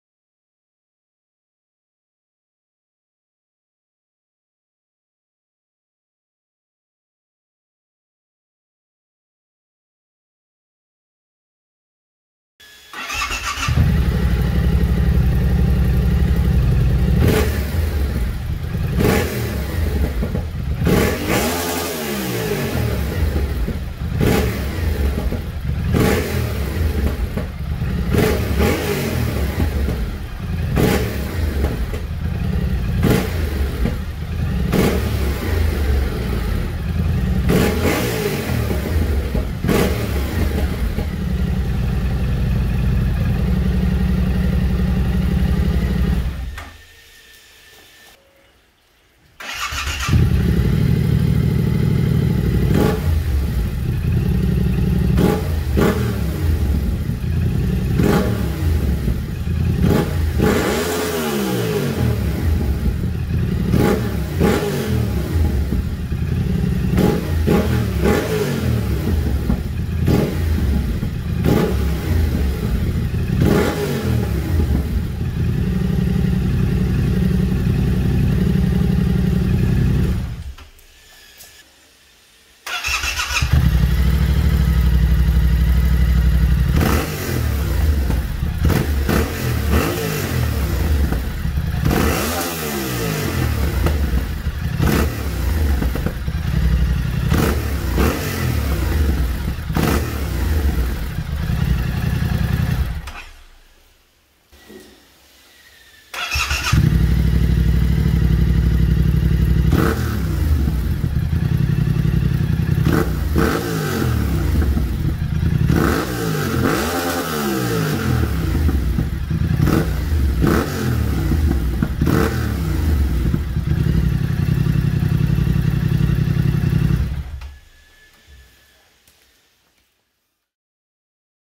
Tracer700 2020 Leovince DB-killer test
Testing the sound of the Leovince Underbody on my tracer 700 2020.
What do you like the most?